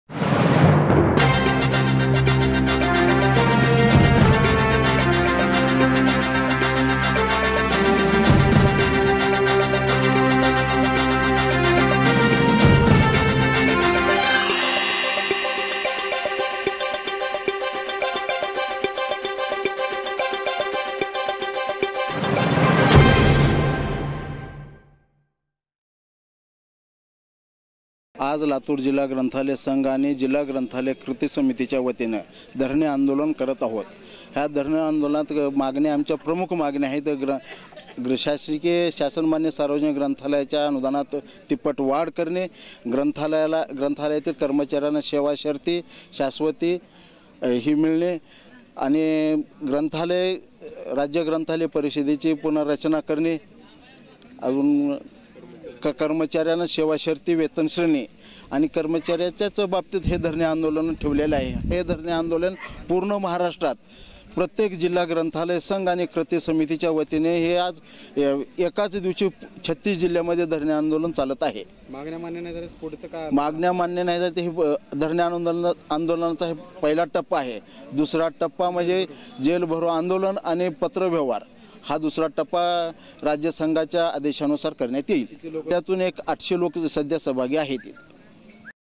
प्रलंबित मागण्यांसाठी ग्रंथालय संघाचे राज्यभर धरणे आंदोलन
लातूर :- महाराष्ट्रातील सार्वजनिक ग्रंथालयाच्या प्रलंबित मागण्यांसाठी आज राज्यभर धरणे आंदोलन करण्यात आले. लातूरच्या जिल्हा सार्वजनिक ग्रंथालय संघाने जिल्हाधिकारी कार्यालयासमोर धरणे आंदोलन करीत सरकार विरोधी घोषणा दिल्या. गेल्या चार वर्षांपासून आपल्या प्रलंबित मागण्यांसाठी शिक्षण मंत्री, अर्थ मंत्री आणि मुख्य मंत्र्यांना प्रत्यक्ष भेटून निवेदने देण्यात आली आहेत. आजच्या या आंदोलनात शासन मान्य सार्वजनिक ग्रंथालयातील २१६११ कर्मचारी आणि ८५,००० पदाधिकारी या राज्यव्यापी धरणे आंदोलनात सहभागी झाले होते. अनुदान वाढ देण्यात यावी, कर्मचाऱयांना वेतन श्रेणी नुसार वेतन निश्चिती करून देण्यात यावी, ग्रंथालयातील कर्मचाऱ्यांना कामाचे तास शासकीय नियमानुसार करण्यात यावे, बंद करण्यात आलेल्या सार्वजनिक ग्रंथालयांना शासन मान्यता देण्यात यावी, राज्य ग्रंथालय परिषदेची आणि जिल्हा ग्रंथालय समित्यांची पुनर्रचना करावी आदी मागण्या करण्यात आल्या आहेत. मागण्या मान्य न झाल्यास तीव्र आंदोलन छेडण्याचा इशारा जिल्हा सार्वजनिक ग्रंथालयाचे कार्यवाह राम मेकले यांनी दिला आहे. या आंदोलनात त्र्यंबकदास झंवर, प्रभाकर कापसे, युवराज जाधव, अशोक खोडवे, पांडुरंग आडसुळे, राम मोतीपवळे, बाळ होळीकर, जहांगीर सय्यद, हावगीराव बेरकिळे, श्रीधर स्वामी, एल. बी. आवाळे, काशिनाथ बोडके, गुप्तलिंग स्वामी, अनिल पाटील, किरण बाभळसुरे, उषाबाई गारटे, आत्माराम कांबळे, यशवंत कुलकर्णी, नंदकुमार घोगरे,